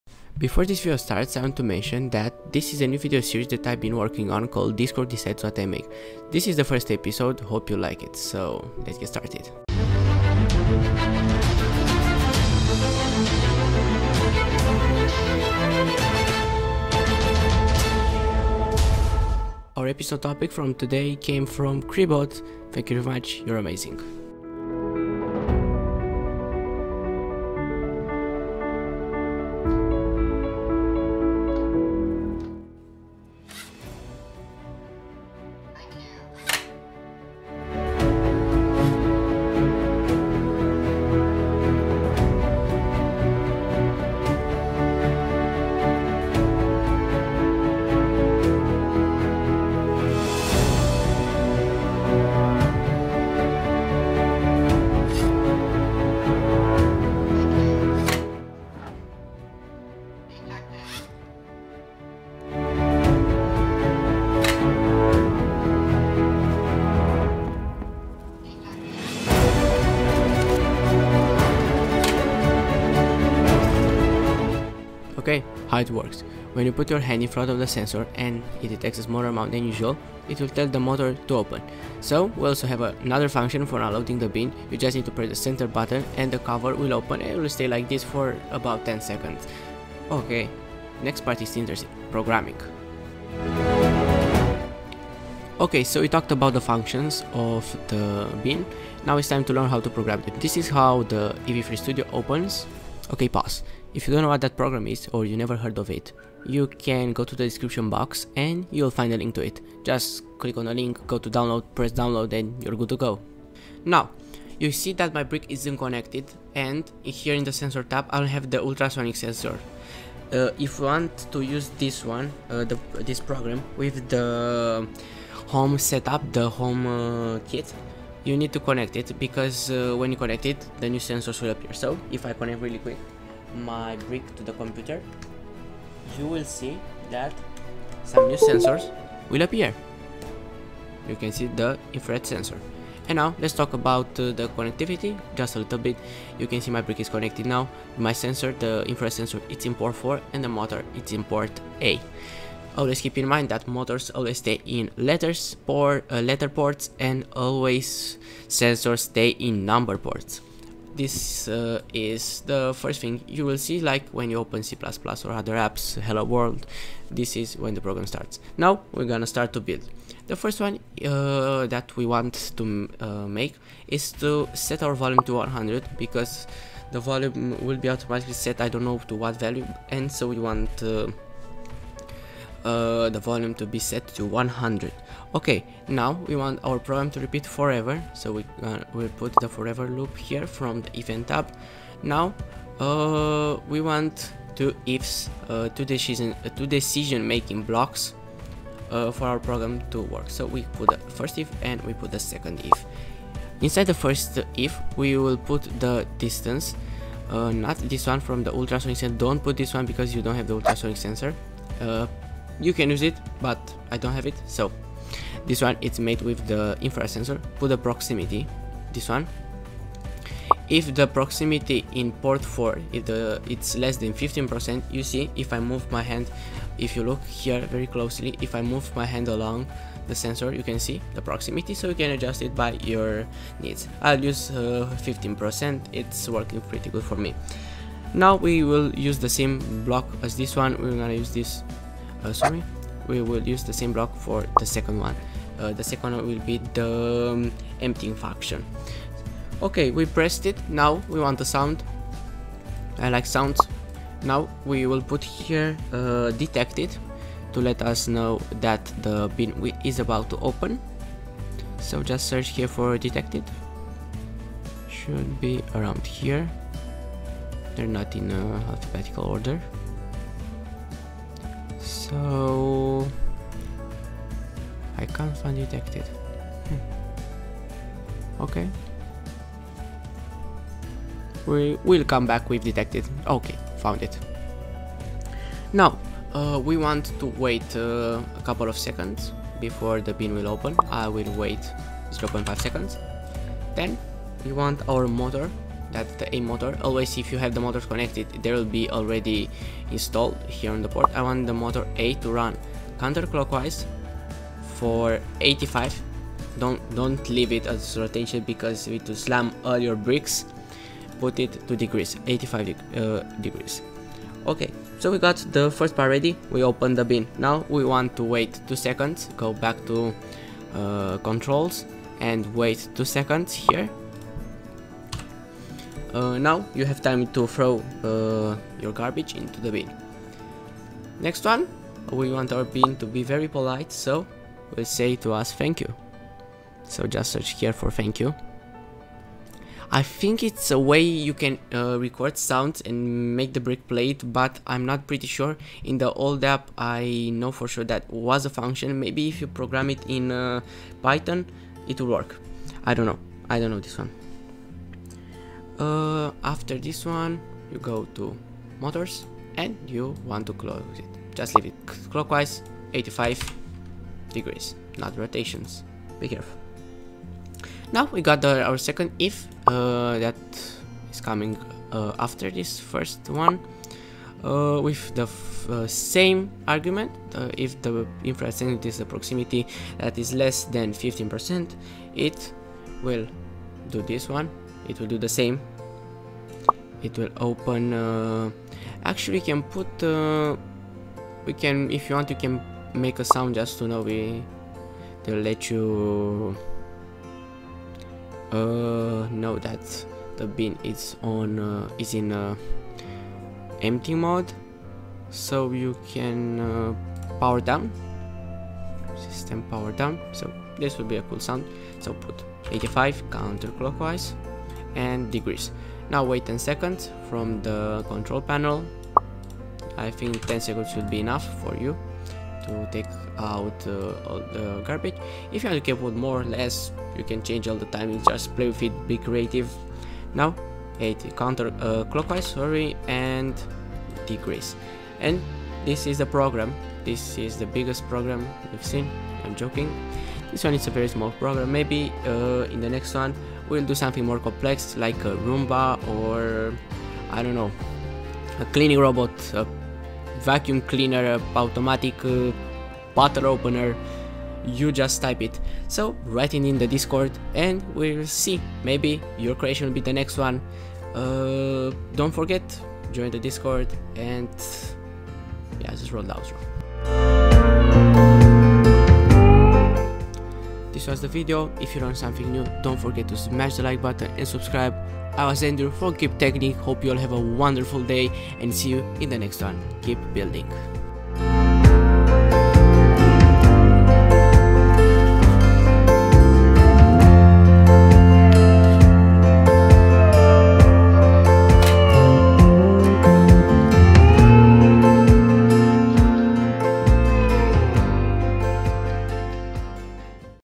LEGO Smart Dustbin w/ Instructions
This is the first episode from this new series "Discord decides what I build". If you haven't joined the discord server now it's time

I will listen to your suggestions about video ideas, just leave them below or on the Discord server.